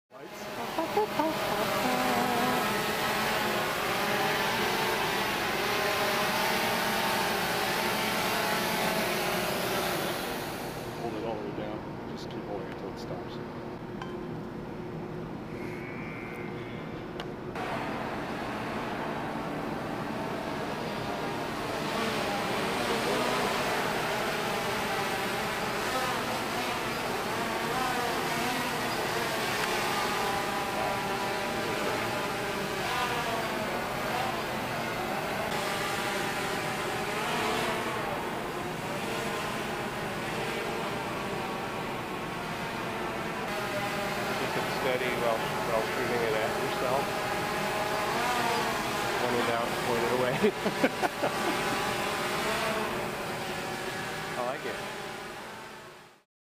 DJI Phantom 2 Vision Quadcopter with Integrated FPV Camcorder
Newest version of DJI Phantom II 2014 flying at flyingym.com Chicago.
The quad has extended range, real time video downlink (0.5 delayed), onboard HD camera.
The quad is capable of altitude hold, position hold and other GPS based modes.
It has been built with the videographer in mind.
Easy to use even for absolute beginners.
Its self leveling functions work very well.
This is the newest model (2014).